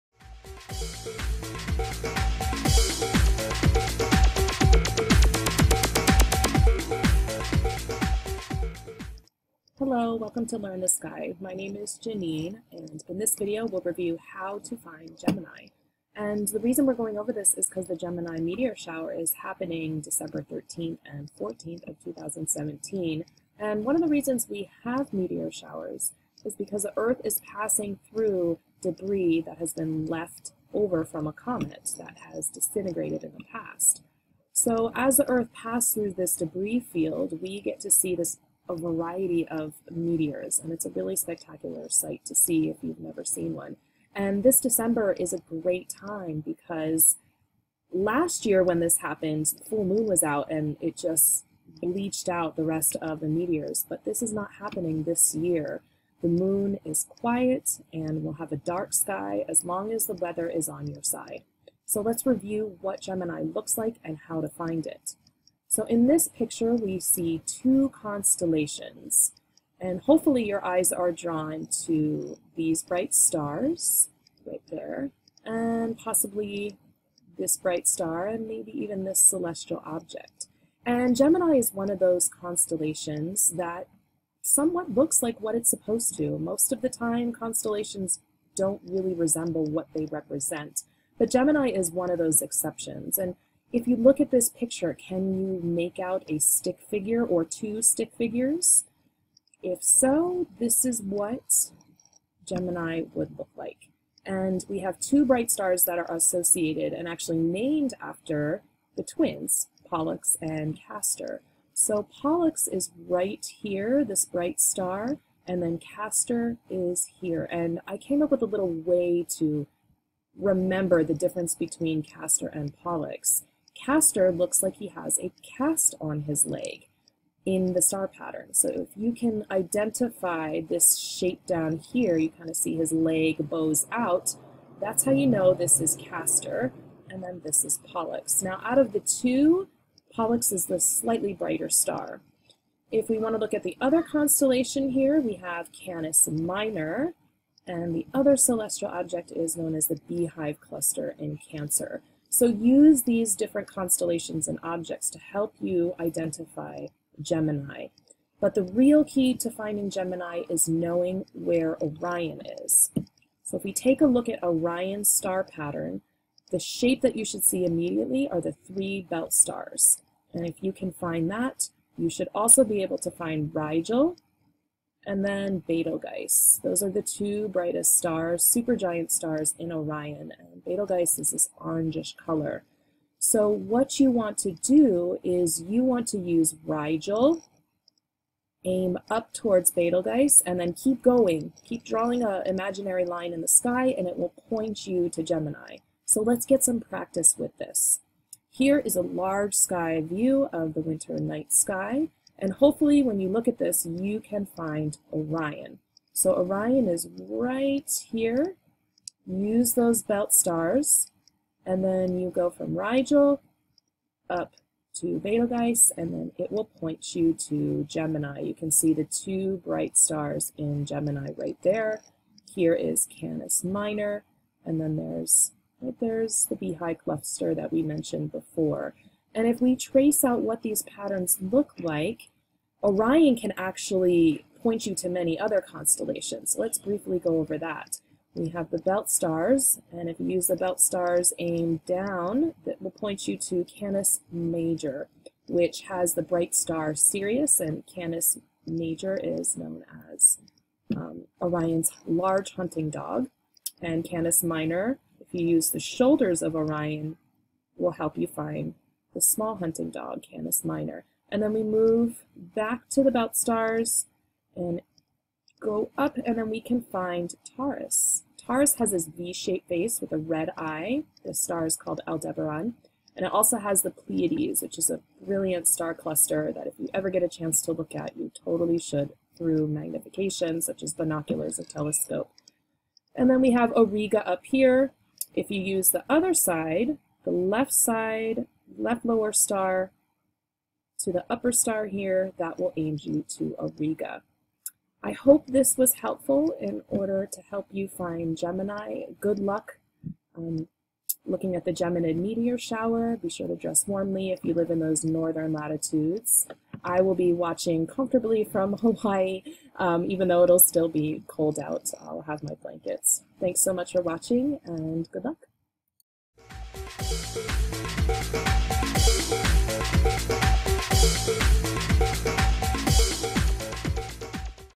Finding Gemini for the Geminid Meteor Shower of 2017
The Geminid Meteor Shower of 2017 is happening from December 13 to December 14 and it is predicted to be a spectacular celestial event.  Review how to find Gemini quickly using the stars of Orion to guide your way.

Chapters
00:00 Introduction
01:03 Review of Gemini
01:46 Pollux and Castor
02:25 Canis Minor & The Beehive Cluster
02:42 Using Orion to Find Other Constellations
04:56 Conclusion